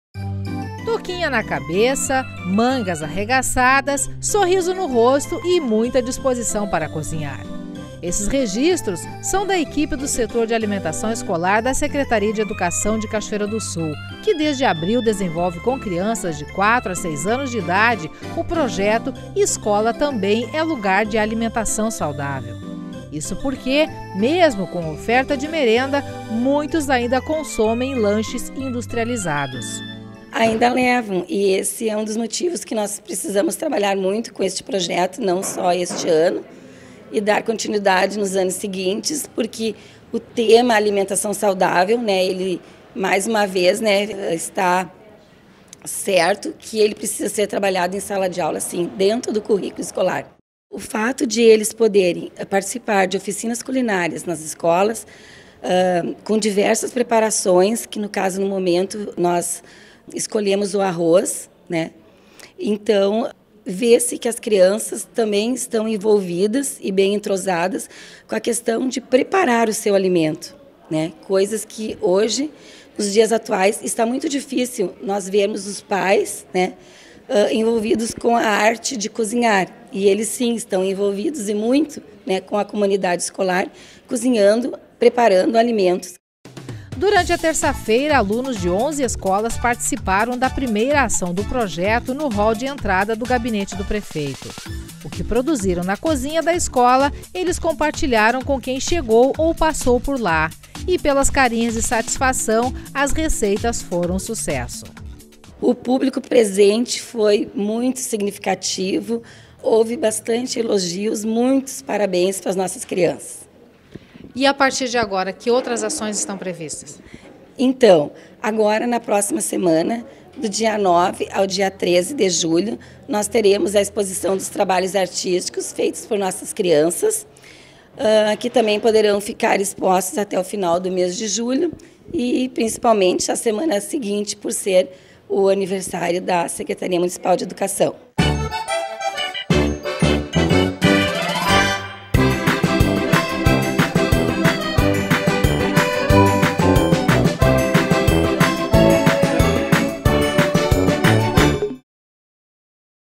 Escola também é lugar de alimentação saudável
Projeto é desenvolvido pela Secretaria de Educação de Cachoeira do Sul com crianças de 4 a 6 anos de idade de 11 escolas do município.